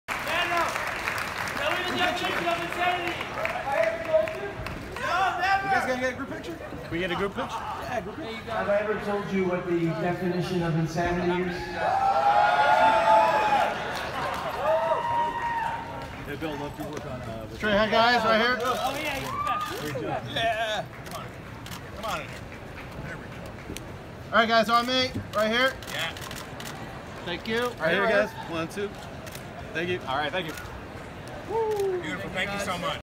Better Call Saul cast and creator at Hall H
Peter Gould, Vince Gilligan, Bob Odenkirk, Rhea Seehorn, Michael Mando, Patrick Fabian and Giancarlo Esposito press group photo at Hall H during the Better Call Saul panel